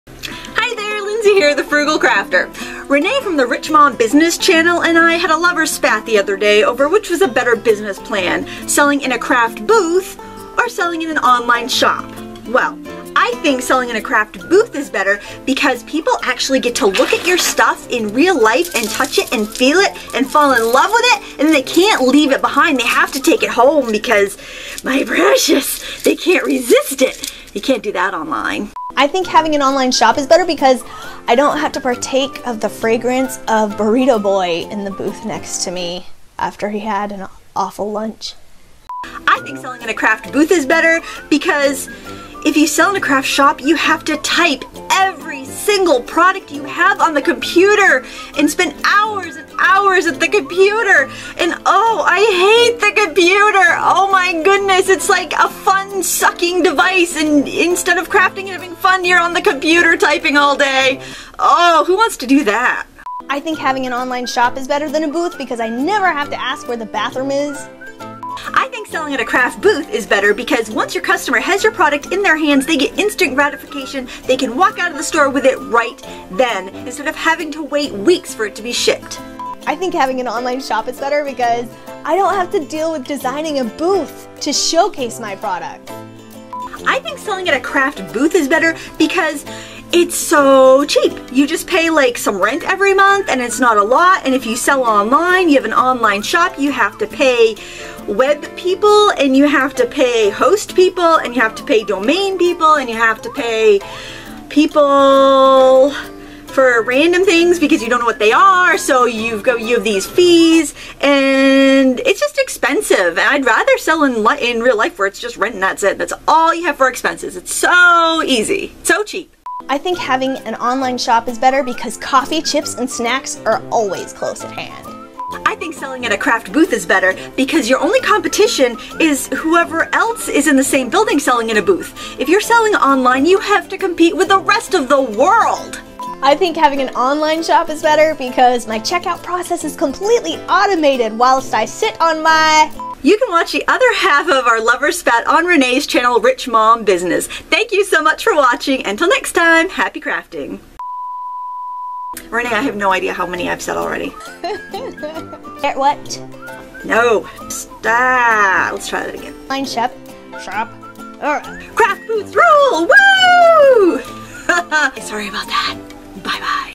Craft Booth vs. Online Shop {With Renae Christine}
My friend Renae from Rich Mom Business and I had a friendly argument over what was a better business plan for a handmade business, selling at a craft booth or an online shop. You can see the rest of the discussion on her channel

So, what do you think? Which is better, selling in a craft booth or online? Happy crafting!